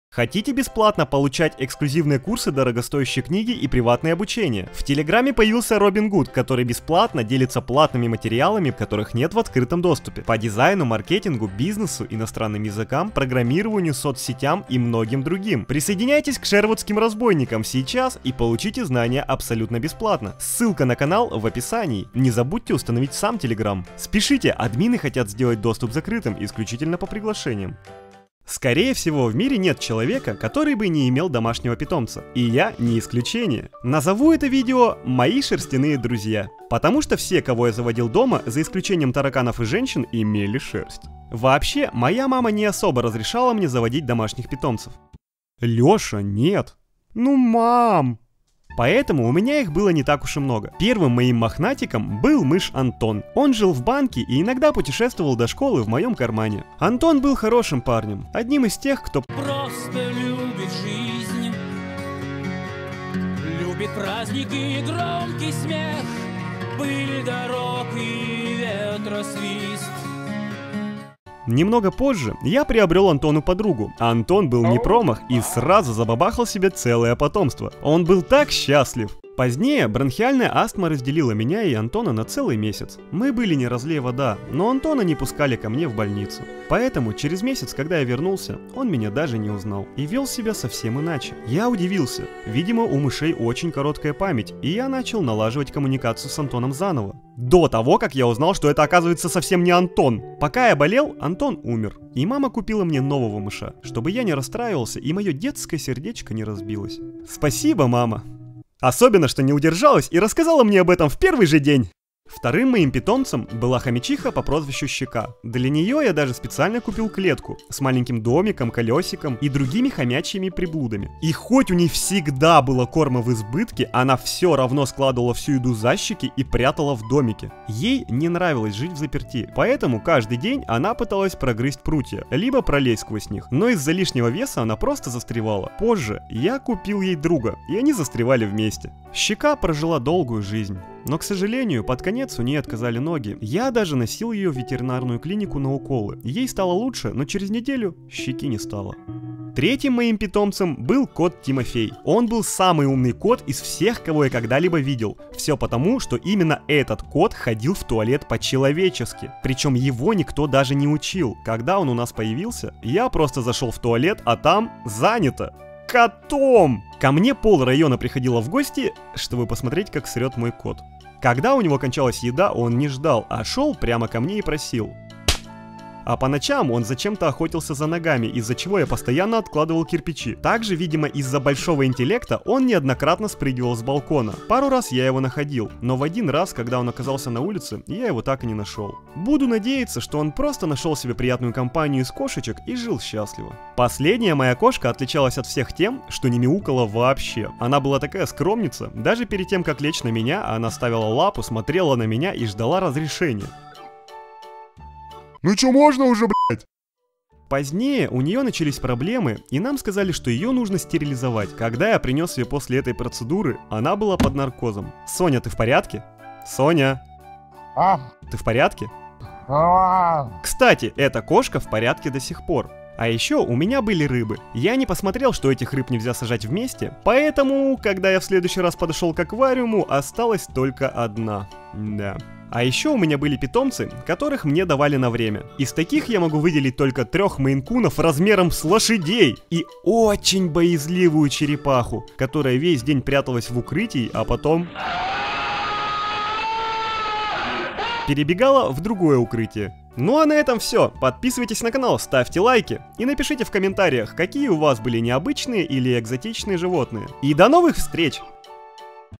МОИ ШЕРСТЯНЫЕ ДРУЗЬЯ (анимация)
- Платное теперь БЕСПЛАТНО! Вступай в Шервудскую банду прямо сейчас и получай знания даром. Доступ вскоре станет закрытым - поторопитесь!

Вспоминал всех своих питомцев, хомяков, котов там всяких и решил сделать мульт , про домашних животных.